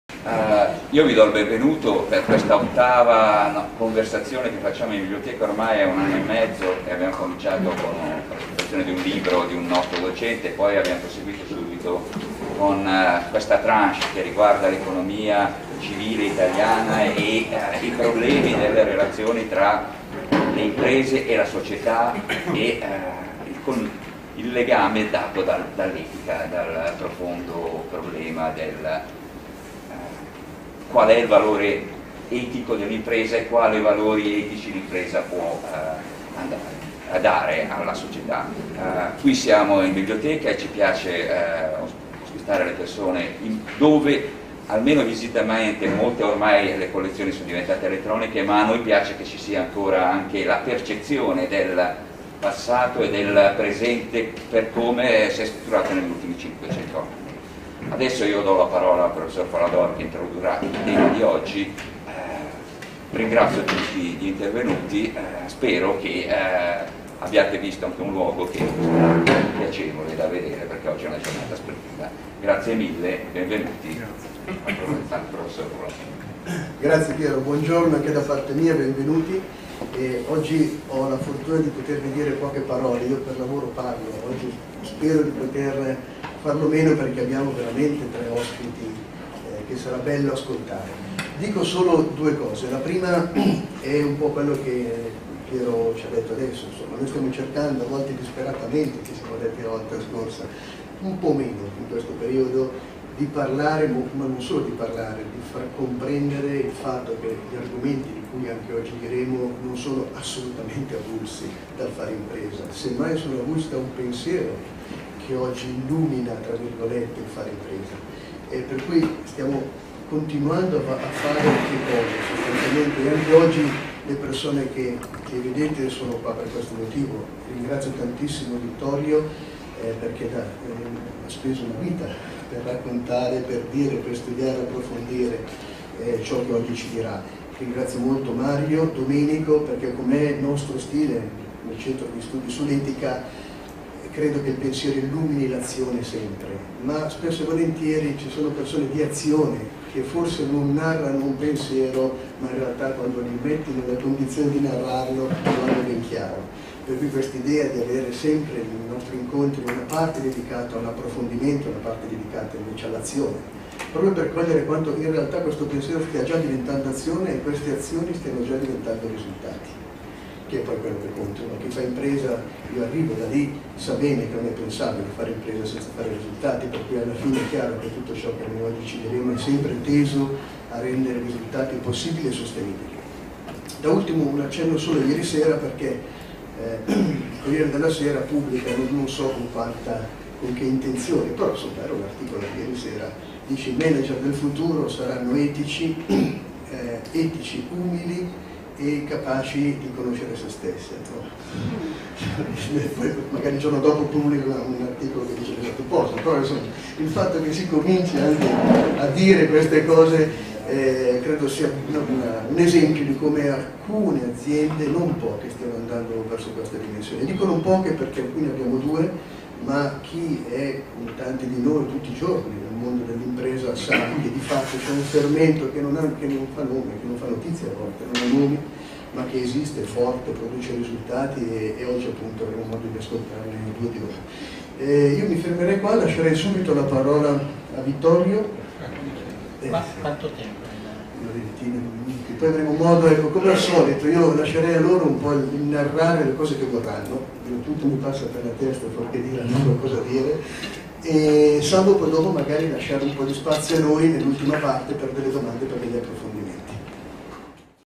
La sfida dell'economia civile - Massimo Folador
La sfida dell'economia civile: buon governo d'impresa e valori morali al servizio del bene comune

Sabato 14 giugno 2014
ore 10.00
LIUC - Università Cattaneo
Biblioteca Mario Rostoni

L'economia civile da sempre vive l'impresa come struttura cardine della società, realtà in grado di dare e ricevere dalla stessa le motivazioni e gli elementi per generare uno sviluppo economico sostenibile.

È proprio dall'incontro tra i valori morali presenti nella società e quelli propri dell'impresa che possono nascere idee, decisioni e azioni che conducono al bene comune.

Intervento di:
Massimo Folador - Direttore Unità di Studi sull'E.T.I.C.A - LIUC - Università Cattaneo